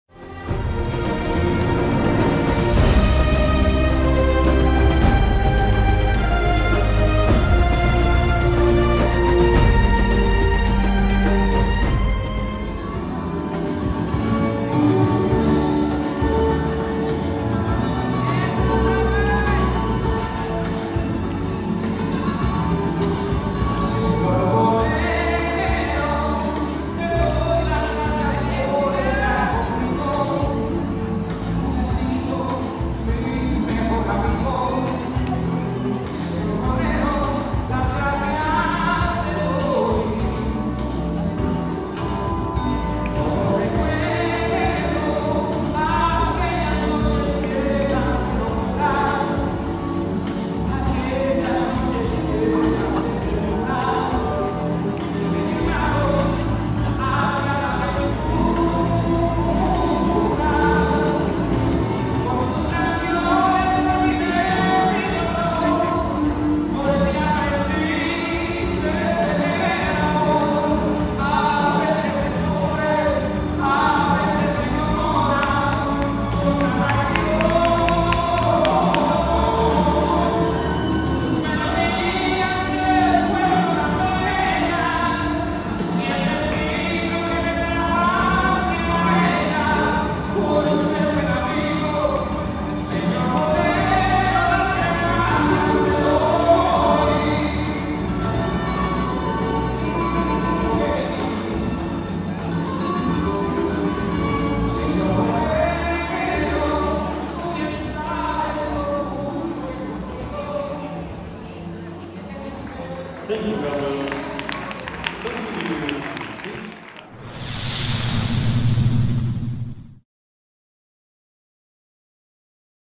Felici - Van Biesbroeck, FRA | 2013 WORLD U-21 LAT R2 R
Lucas Felici - Amandine Van Biesbroeck, FRA, dance the Rumba in the second round of the 2013 World U-21 Latin in Platja d'Aro, ESP.